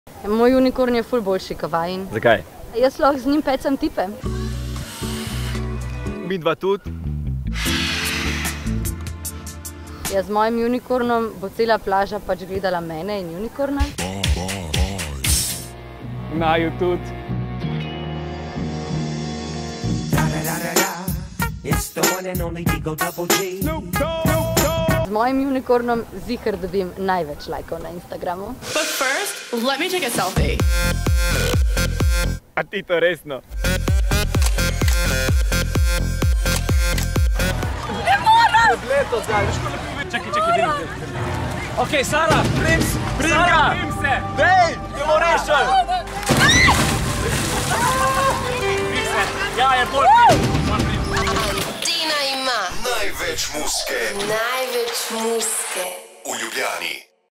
Antenin floatie battle na Bledu
Antenina ekipa je stestirala hit tega poletja - FLOATIE. Velika napihljiva blazina v obliki različnih živali je preplavila Instagram, zato smo preizkusili še enega posebnega - napihljivi penis. Kateri ti je boljši? :D